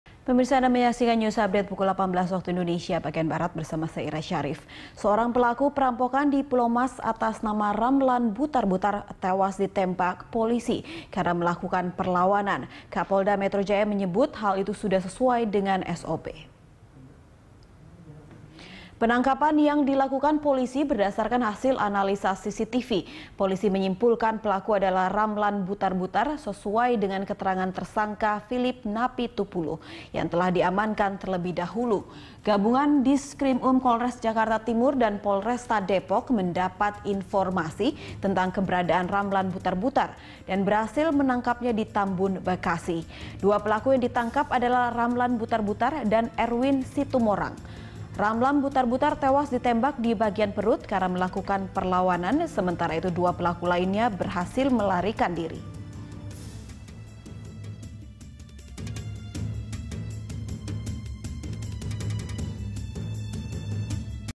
1 Pelaku Pembunuhan Sadis di Pulomas Tewas Ditembak
Seorang pelaku perampokan di Pulomas atas nama Ramlan Butarbutar tewas ditembak polisi karena melakukan perlawanan. Kapolda Metro Jaya menyebut hal itu sudah sesuai dengan SOP.

Facebook.com/BeritaSatuTV
Youtube.com/BeritaSatu